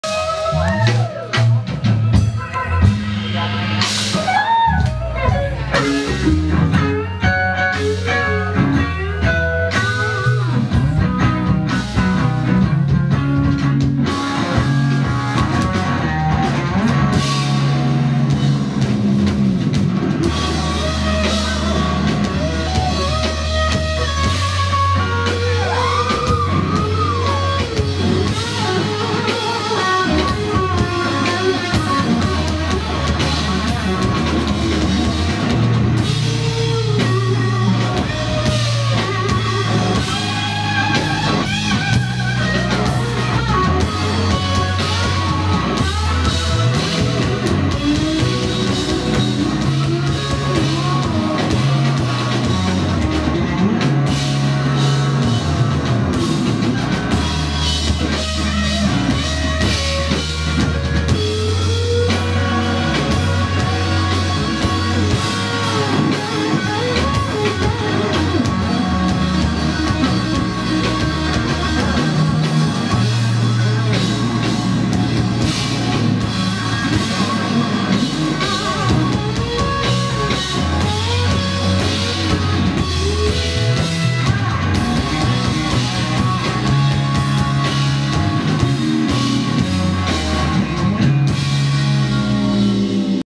ERIC TESSMER BAND
My first time seeing the Eric Tessmer Band at Dan Electro's Guitar Bar